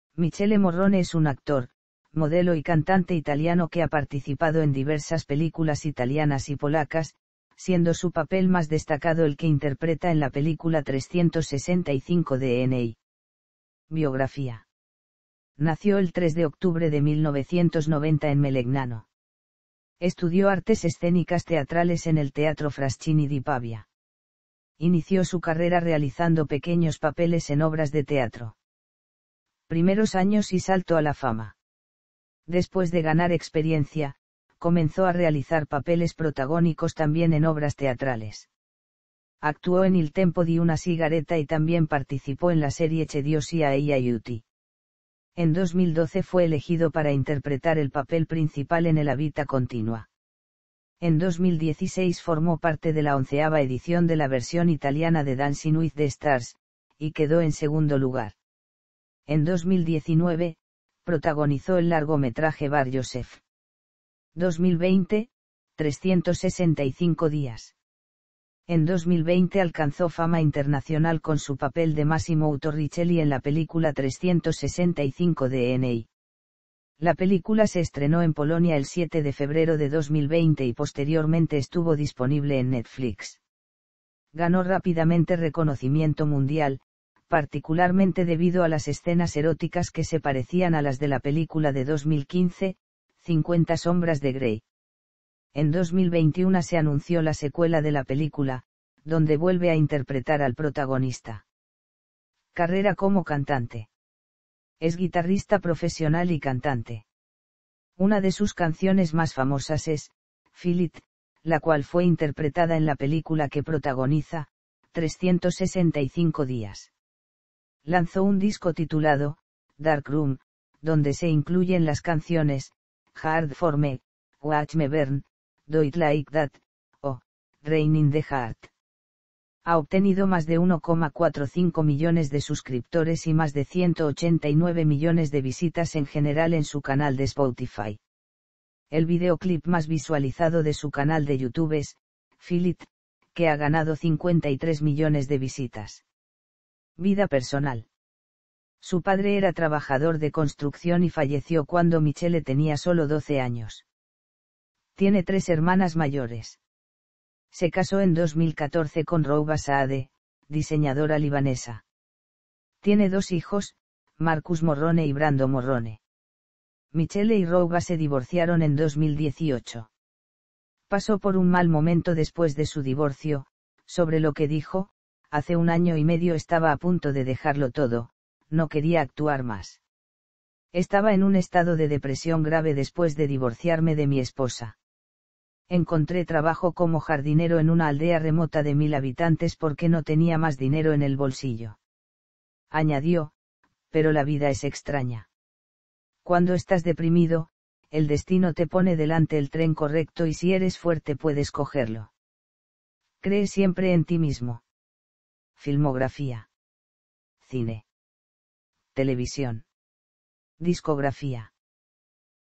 Michele Morrone - Narrated Wiki español
Michele Morrone es un actor, modelo y cantante italiano que ha participado en diversas películas italianas y polacas, siendo su papel más destacado el que interpreta en la película 365 Dni.